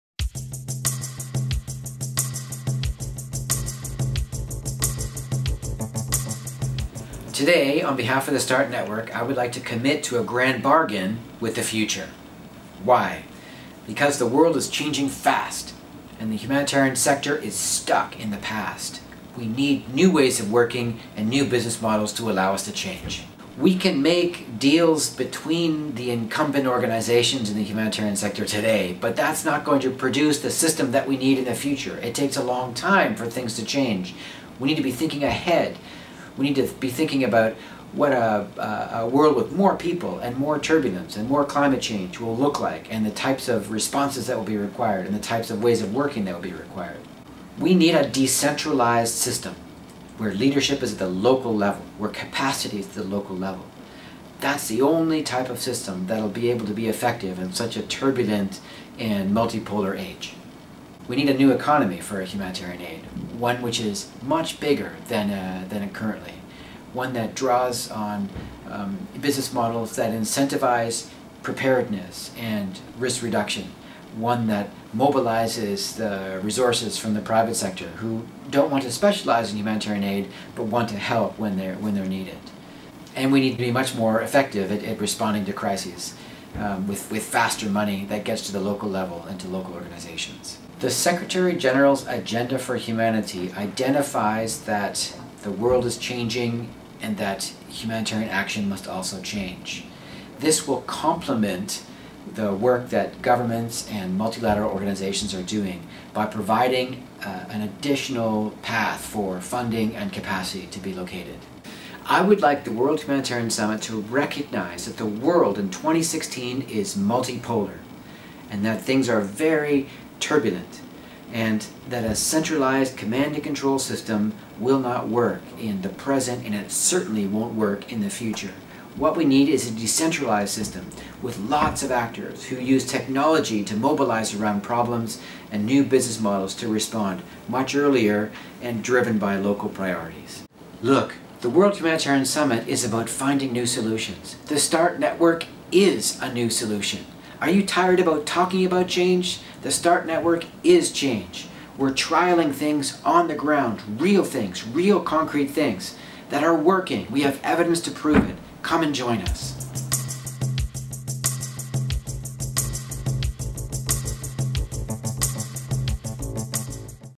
Sean Lowrie on the Grand Bargain with the Future
In a more turbulent and uncertain world, people everywhere desire the ability to respond to the suffering of their neighbours in times of humanitarian crises. Today’s escalating and changing humanitarian needs demand a much bigger, whole-of-society approach.  A common NGO position for the Grand Bargain at the World Humanitarian Summit would describe a 21st Century humanitarian system which would enable every humanitarian to make a contribution.